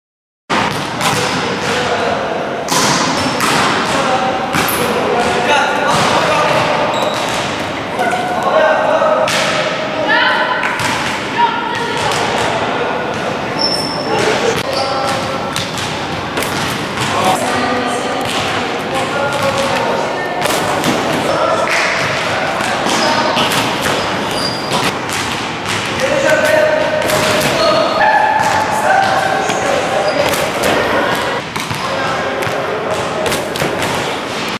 Emre Tanyeli yüksek atlama TAC highjump athletics tarsus amerikan koleji atletizm
TAC öğrencisi Emre Tanyeli'nin yüksek atlama çalışması